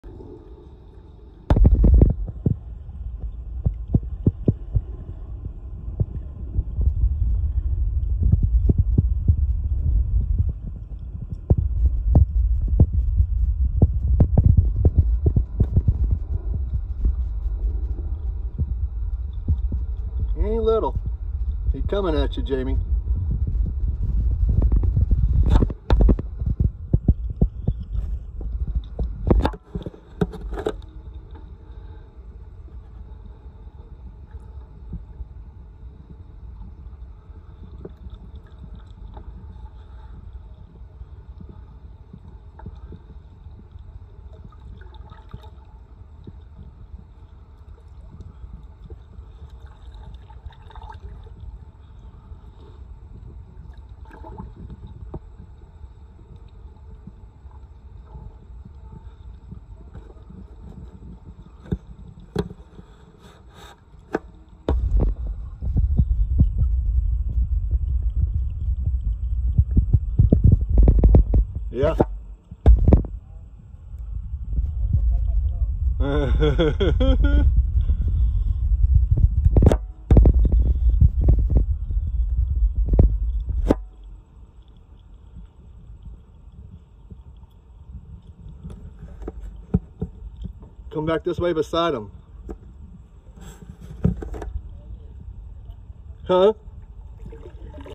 Floating down river with a Big Gator! 🐊
We thought it was following us at first, but I guess it was just going down river also ha ha. Wipes brow

Look out Jamey it’s coming at you! Lol